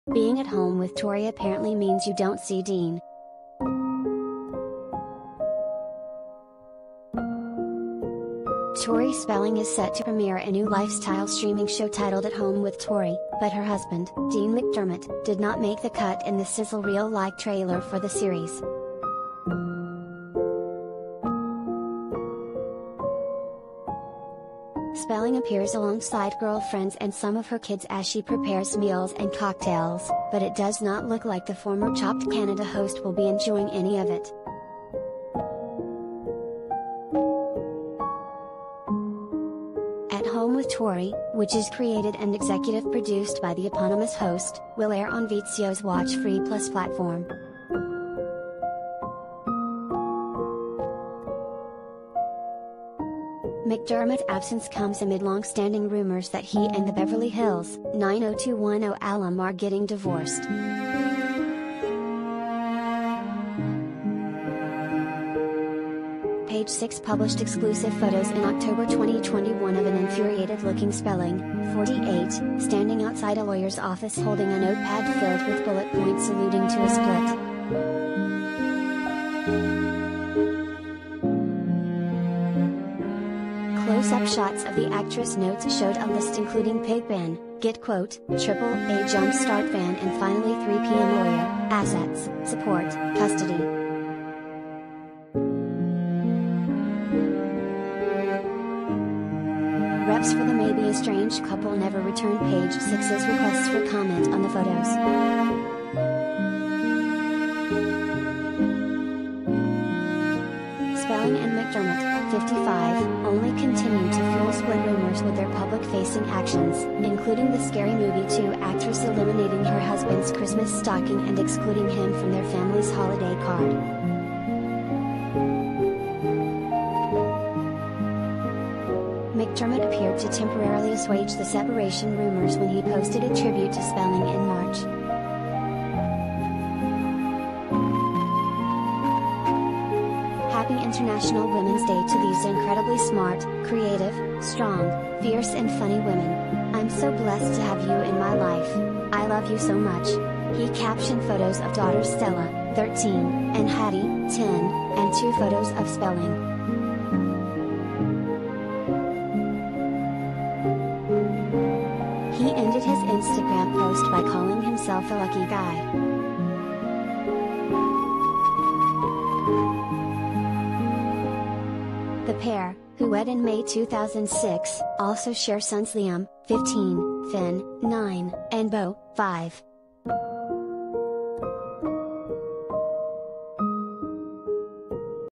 Dean McDermott absent from Tori Spelling’s ‘@Home with Tori’ trailer
Being “@Home with Tori” apparently means you don’t see Dean.
Tori Spelling is set to premiere a new lifestyle streaming show titled “@Home with Tori,” but her husband, Dean McDermott, did not make the cut in the sizzle reel-like trailer for the series.
Spelling appears alongside girlfriends and some of her kids as she prepares meals and cocktails, but it does not look like the former “Chopped Canada” host will be enjoying any of it.
“@Home with Tori,” which is created and executive produce